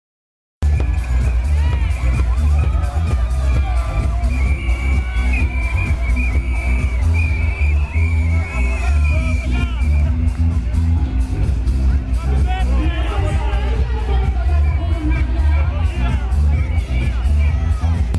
1o Καλαματιανό Καρναβάλι
Το 1ο Καλαματιανό Καρναβάλι είναι γεγονός!
Περίπου 3.000 μικροί και μεγάλοι καρναβαλιστές  συμμετέχουν στην μεγάλη παρέλαση στον κεντρικό δρόμο της πόλης.